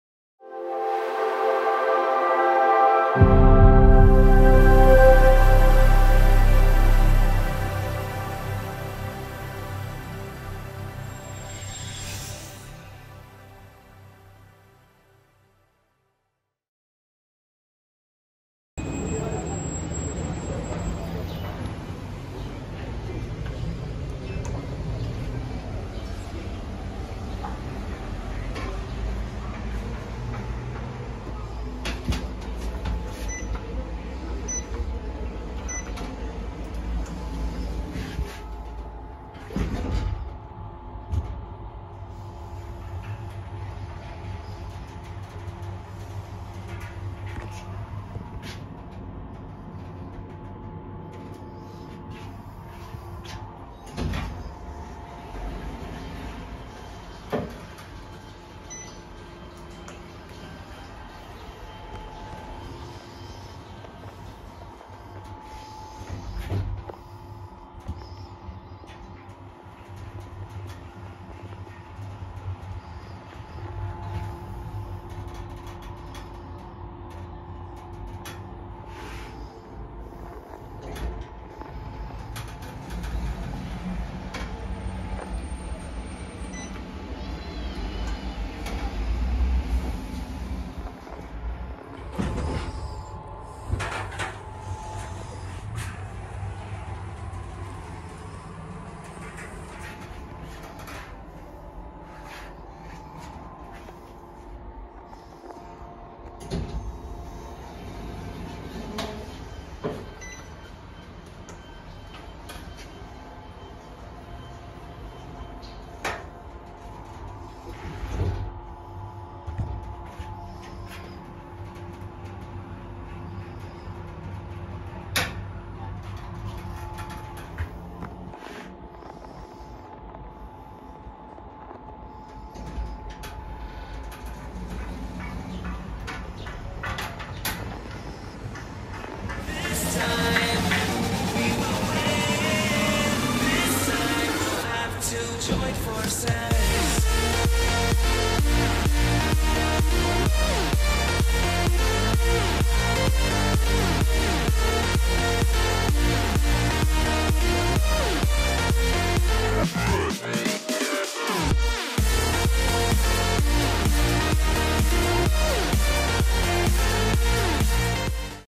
2019 OTIS GEN2 MRL traction elevator @ White City Residences, Tel Aviv, Israel.
---Public Parking elevator---
Brand: OTIS
Type: MRL traction
Capacity: 630kg / 8 Persons
Model: GEN2
Fixtures: Ambiance
Installed at 2019
Floor served: 4(^-3^, ^-2^, -1, *0*)
Elevator bank size: 1
Serial number: 46NT6815

Outro thumbanil taken by me.